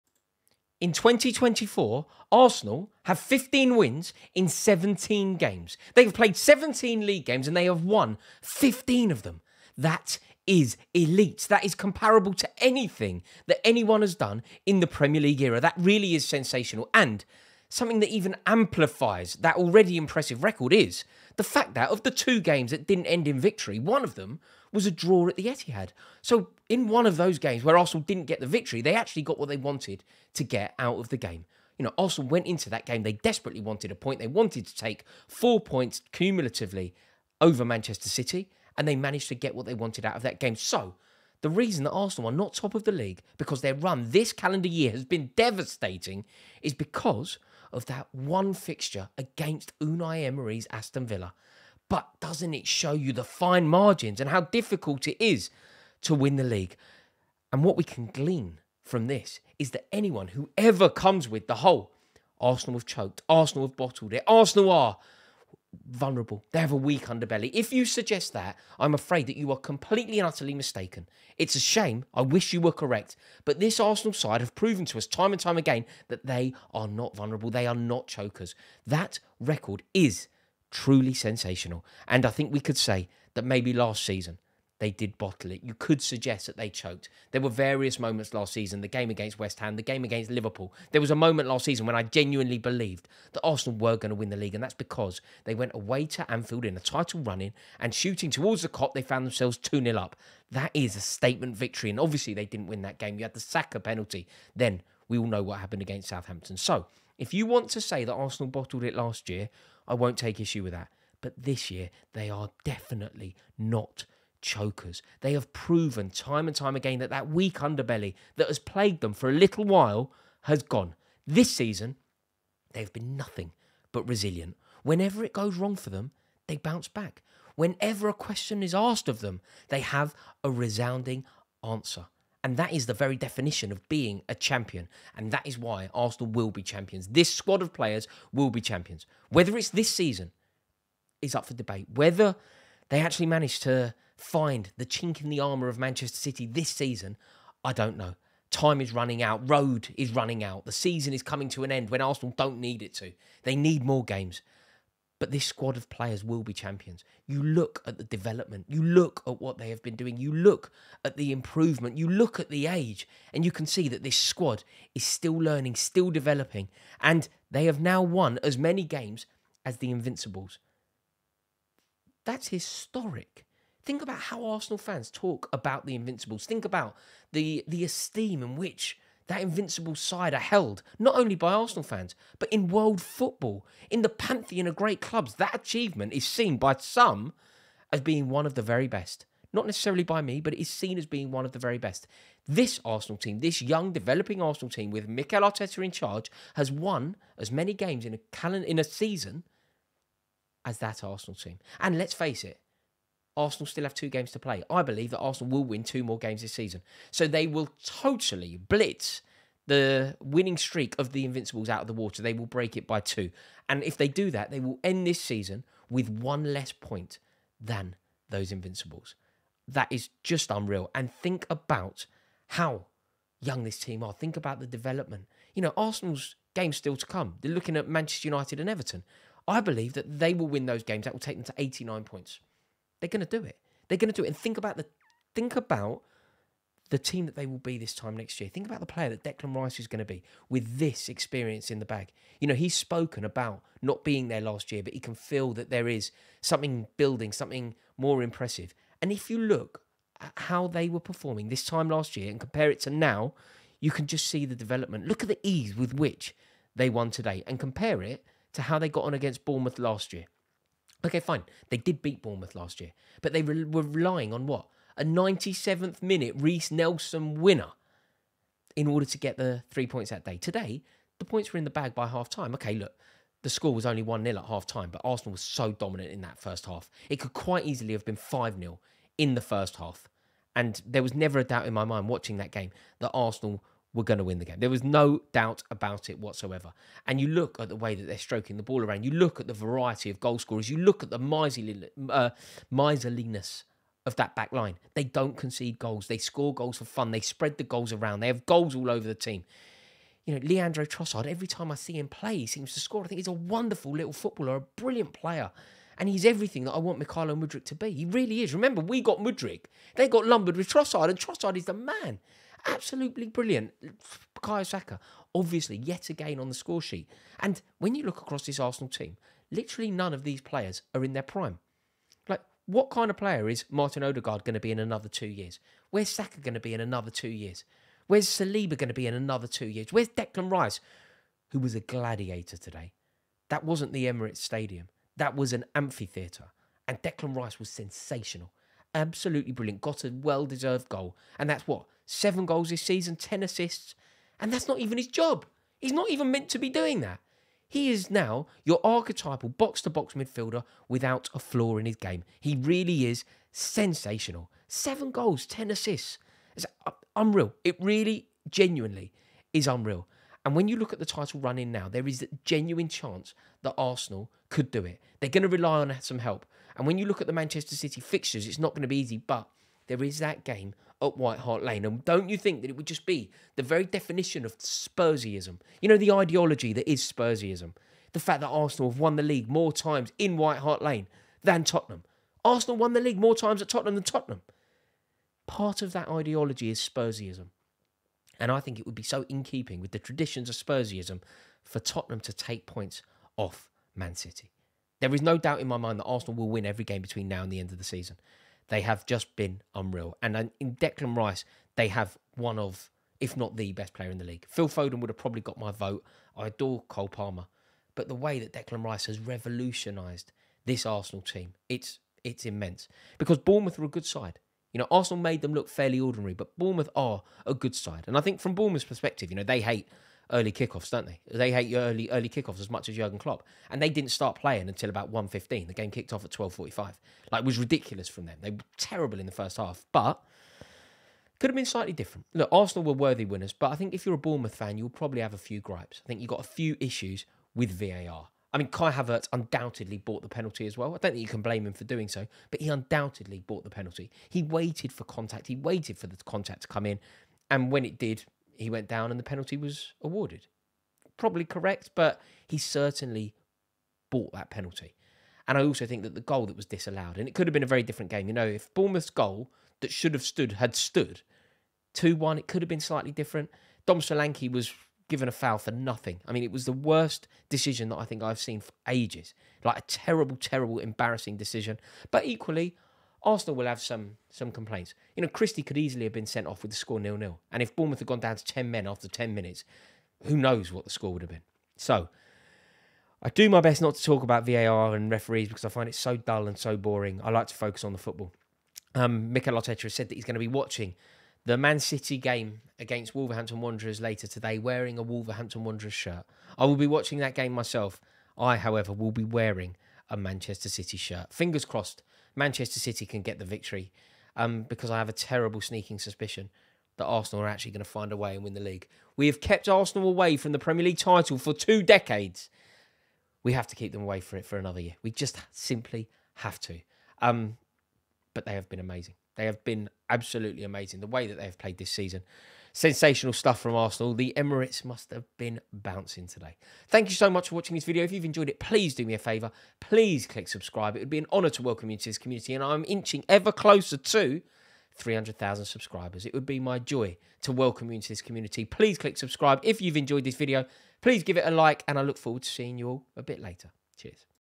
Why Arsenal Will Win The Premier League…
Arsenal currently sit at the top of the Premier League with only two games to play. Saka and Trossard have again scored for Arsenal and it is fair to assume that Arteta's side will now win every single game between here and the end of the season. So it is over to man City and Pep Guardiola to stop them...